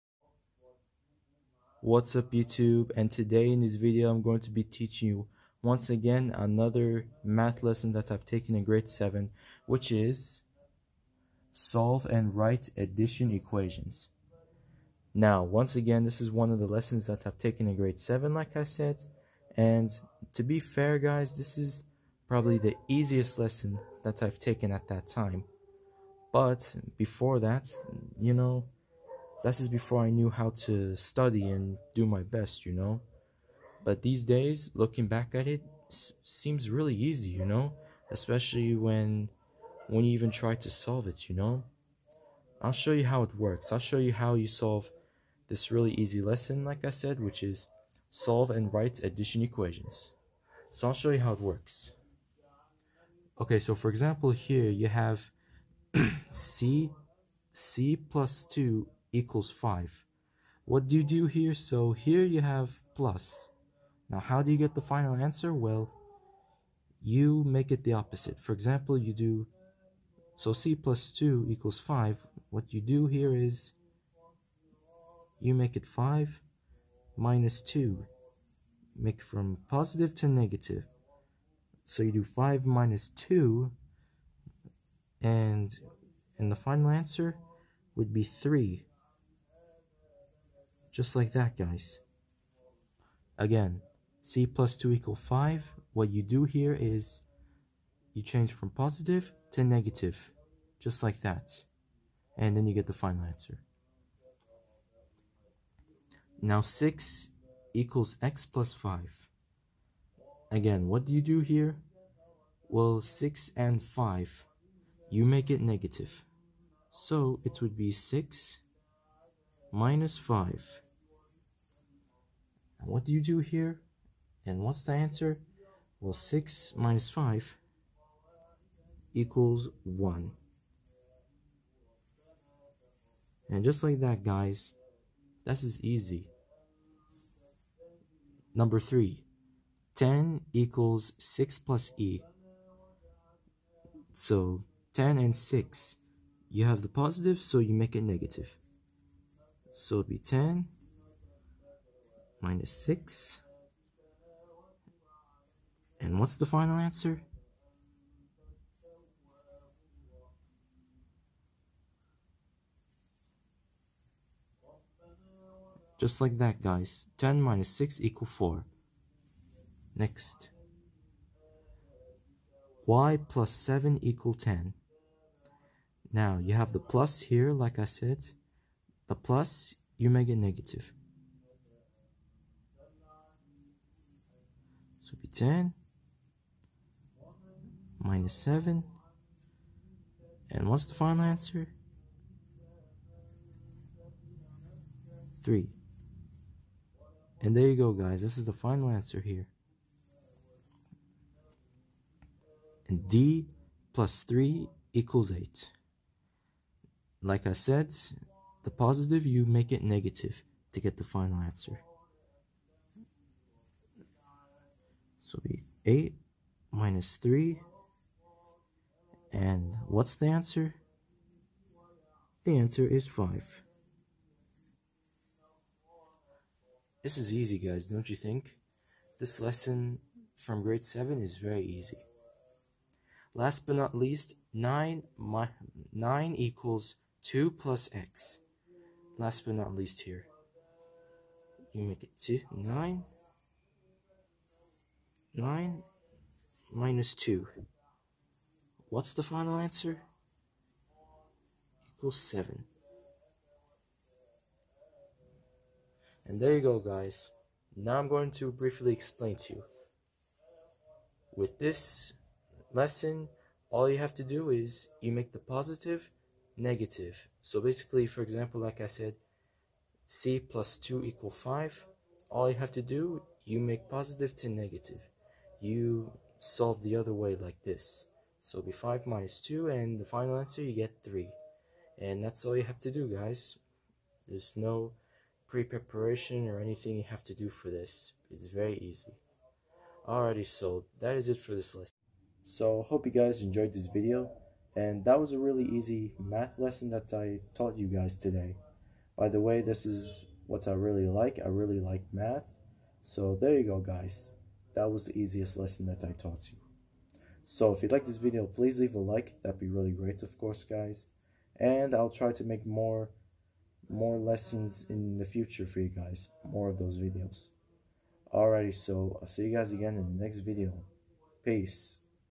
Solve and write addition equations
How to solve:

For example like C+2=5 you solve it backwards like subtract 5-2. Then you get the final answer which is 3. Very easy stuff. I should've studied back then, but now I'm a hard worker who loves to learn in school.